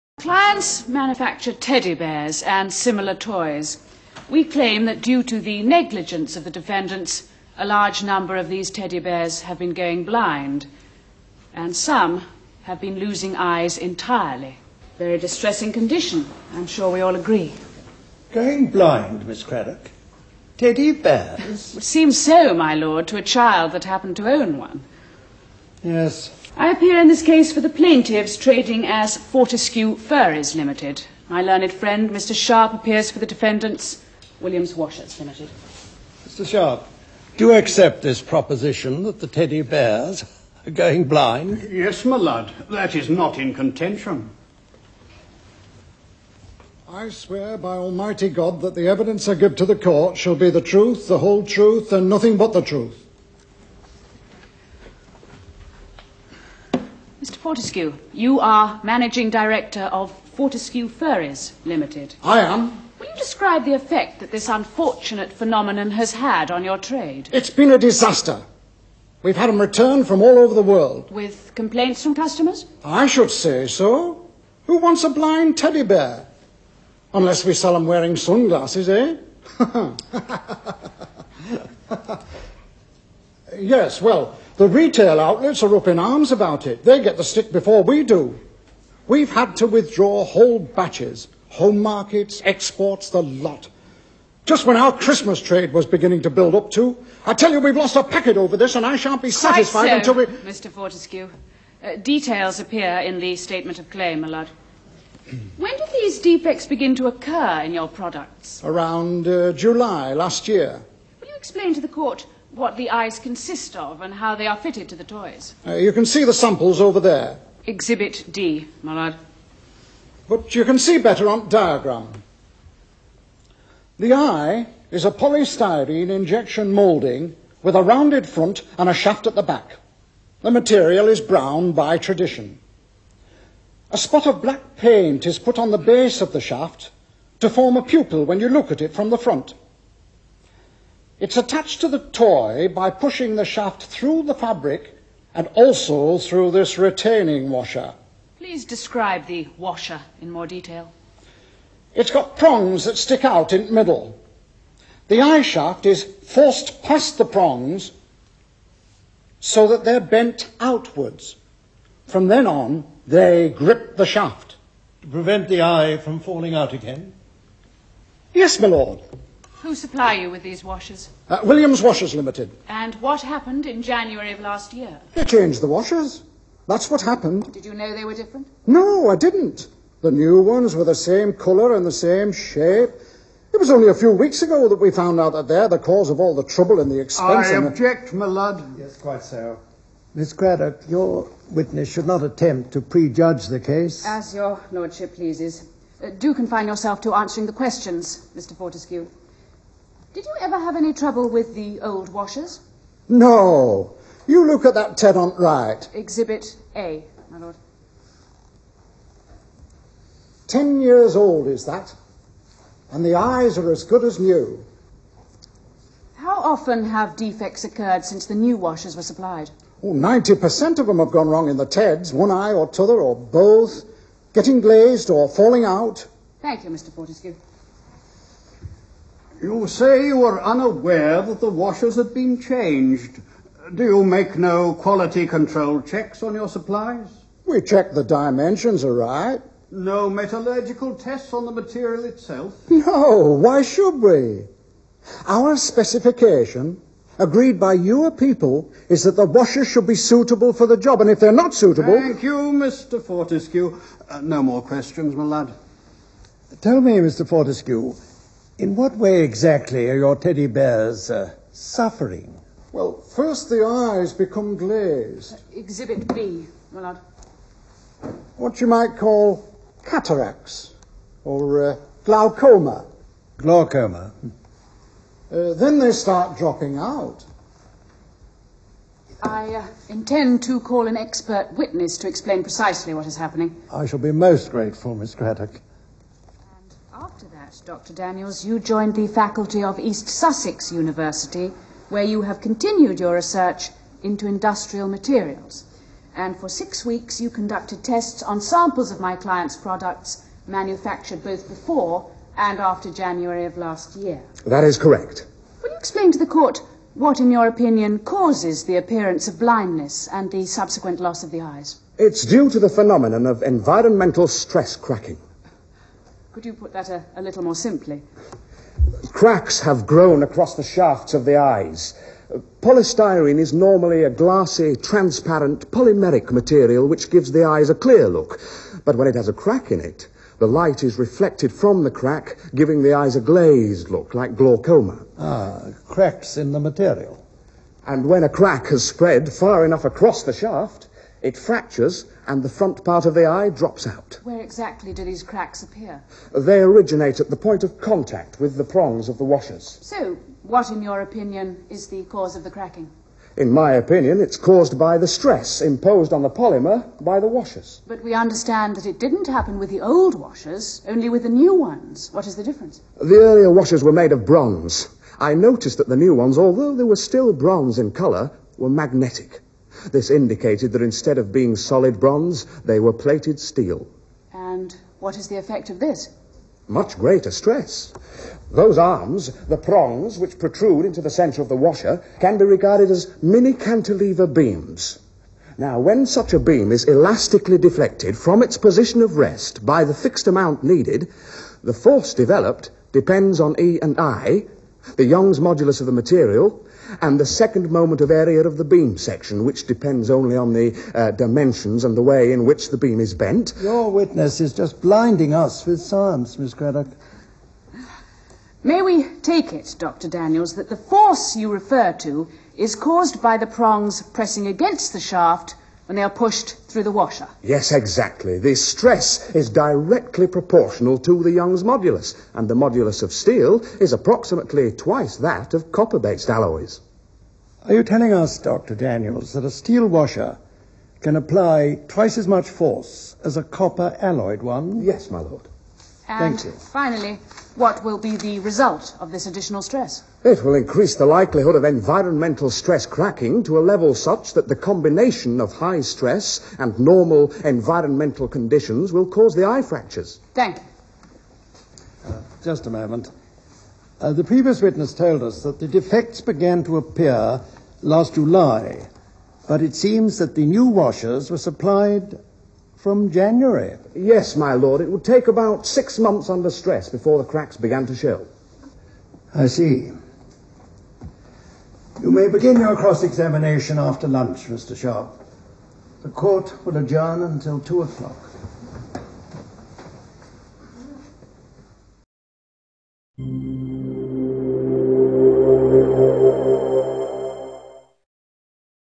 An eye for an eye? (1/3)
A fictional court case, taken from a real case, which involved a failure of materials.